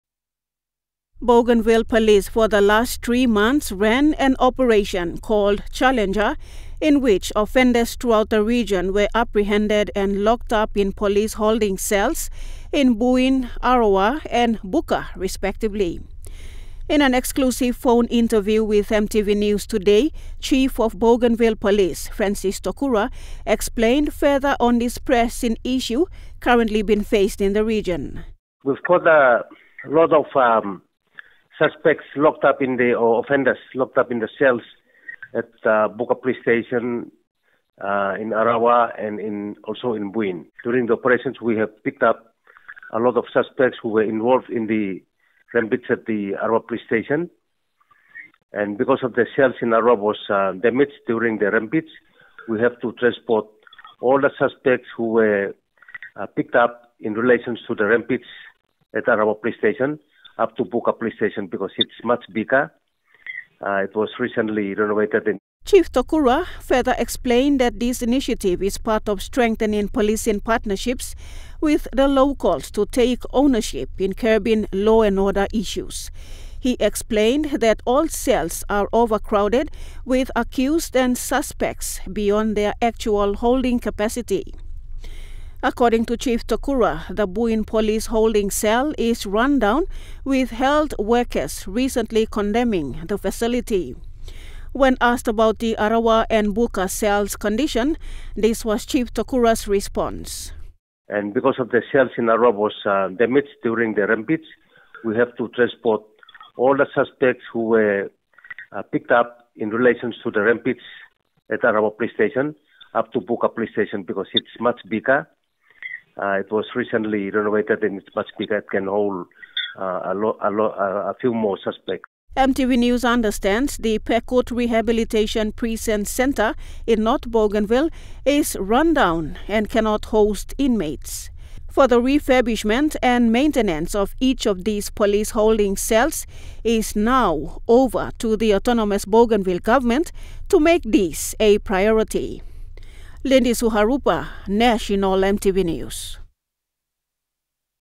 INFLUX OF INMATES CONCERN
Frequent influx of inmates into police holding cells in the Autonomous Region of Bougainville has become a grave concern with local authorities there.
Chief of Bougainville Police Francis Tokura has revealed that all cells in Buin, Arawa and Buka are all full to capacity at this time.